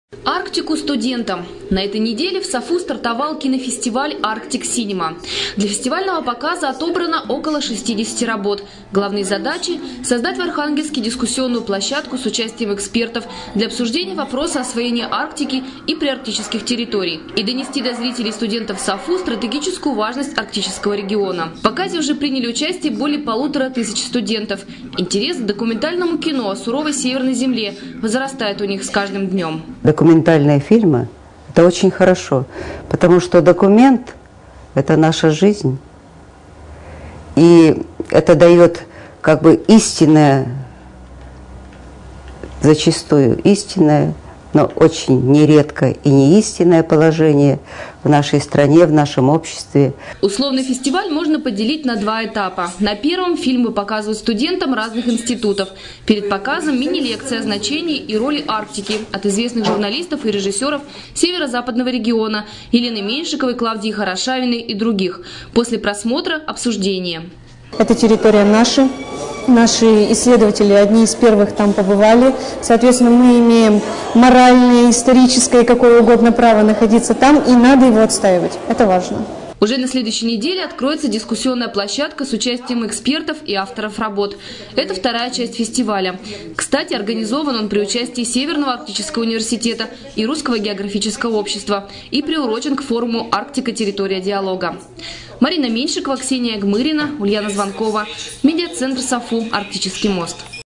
«Arctic Cinema» — успешный проект университета
5 сентября в Архангельске стартовал I Международный фестиваль «Arctic Cinema». На кинофестивале транслируются документальные фильмы об Арктике. Продлится он до 16 сентября.